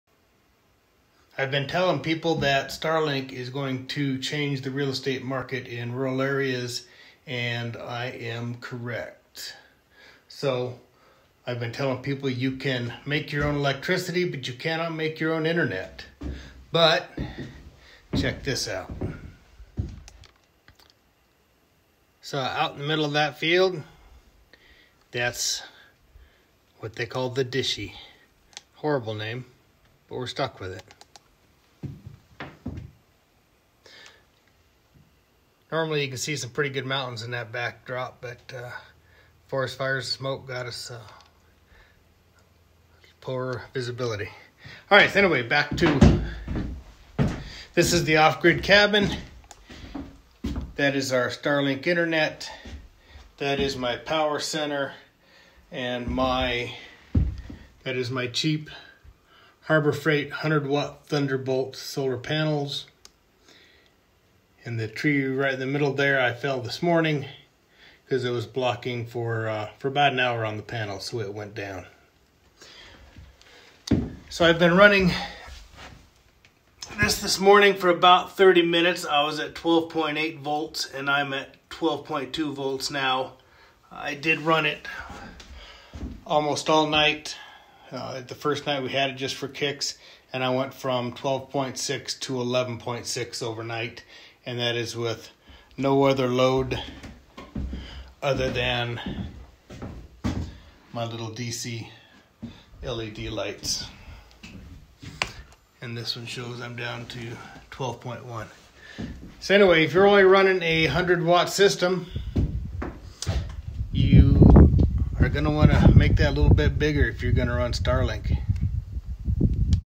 Off-Grid Cabin with Starlink
My experiences with Starlink Internet and 100 Watt solar system.